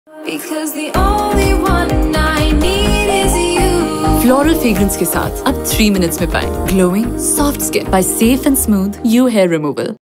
EU TVC
TimetoChange and bring out what's best in you. EUCream offers goodness of soft, smooth skin with SheaButter in just 3Minutes. Celebrate beauty and bring that change in your life as, ChangeStartsWithYou.